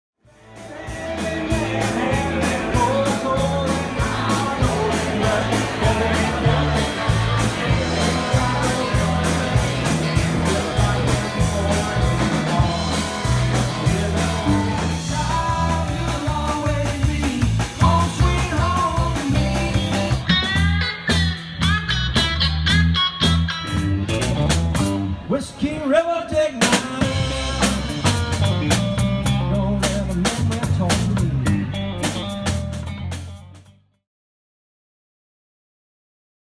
Tallboys Miami Country Rock Promo
70s Country Rock band from Miami, FL.  Willie, Waylon and Skynyrd